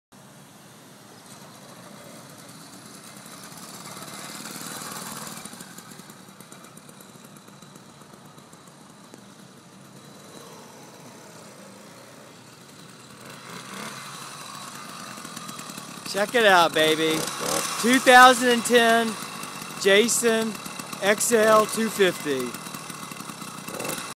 Motor Bicycle
A diamonback wildwood citi with a 48cc skyhawk chinese engine on it